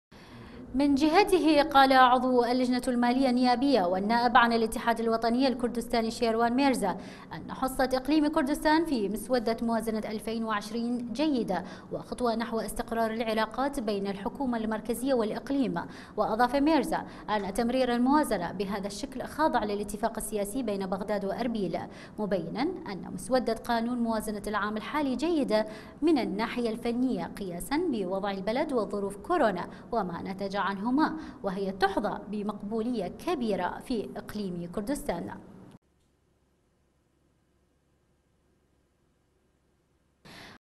ميرزا: موازنة 2020 تحظى بمقبولية كردستان
الاقتصاد العراقي

من جهته , قال عضو اللجنة المالية النيابية، والنائب عن الاتحاد الوطني الكردستاني، شيروان ميرزا ، إن حصة إقليم كردستان في مسودة موازنة الفين وعشرين جيدة وخطوة نحو استقرار العلاقات بين الحكومة المركزية والإقليم , واضاف ميرزا أن تمرير الموازنة بهذا الشكل خاضع للاتفاق السياسي بين بغداد واربيل ، مبينا أن مسودة قانون موازنة العام الحالي جيدة من الناحية الفنية قياسا بوضع البلد وظروف كورونا وما نتج عنهما وهي تحظى بمقبولية كبيرة في إقليم كردستان .